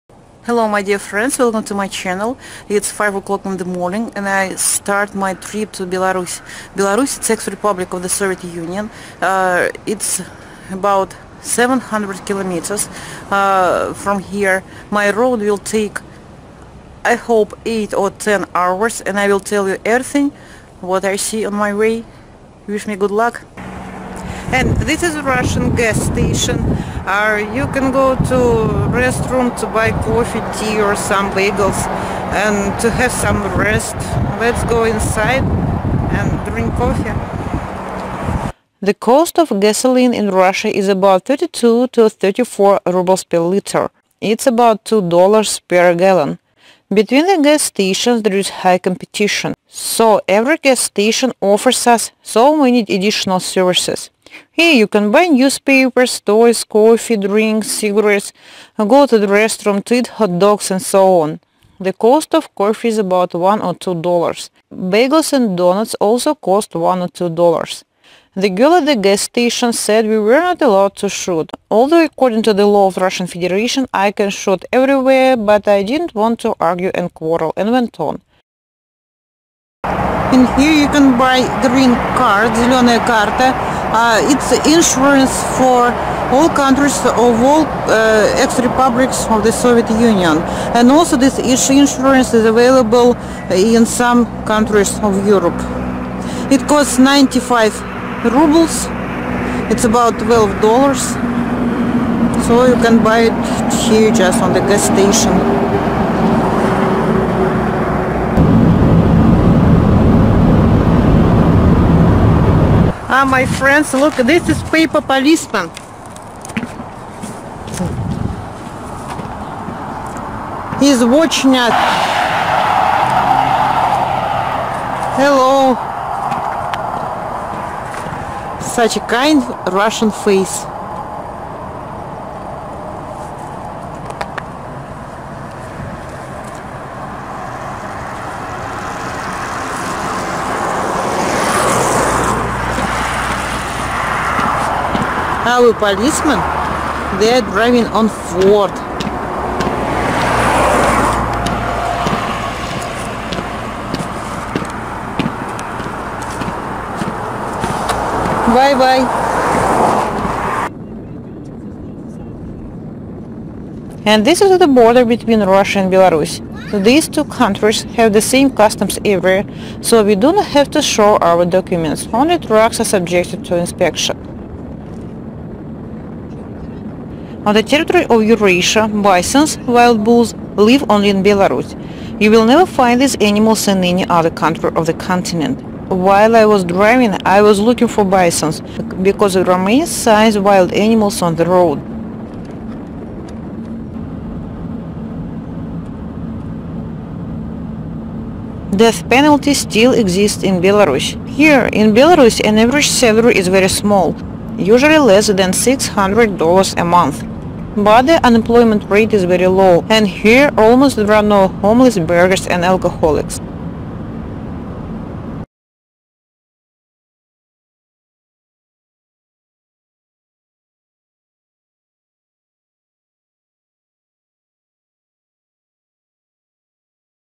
My trip to Belarus.
On the  territory of Eurasia Bison - wild bulls live  only in Belarus. You will never find these animals in any other country of the continent. While I was driving I was looking for bisons, because there are many signs “Wild Animals” on the road. But I saw only paper policemen.
Belarus still applies the death penalty.
 In Belarus, an average  salary is very small (usually less than $ 600 a month), but the unemployment rate is very low, almost there are no homeless, beggars and alcoholics.

The cost of gasoline in Russia is about 32-34 rubles per liter, it is about 2 dollars per a gallon. Between the gas-stations there is high competition, so every gas-station offers us as many additional services, here you can: buy newspapers, toys, coffee, drinks, cigarettes, go to the restroom, to eat hot dogs and so on. The cost of coffee is about  $ 1-2 per glass. Bagels and donuts cost also 1-2 dollars.  The girl at the gas station said that we were not allowed to shoot. Although according to the law of the Russian Federation, I can shoot everywhere, but I did not want to argue and quarrel and went on.
This is the border between Russia and Belarus. These two countries have the same customs area, so we do not have to  show our documents.  Only trucks are subjected to inspection.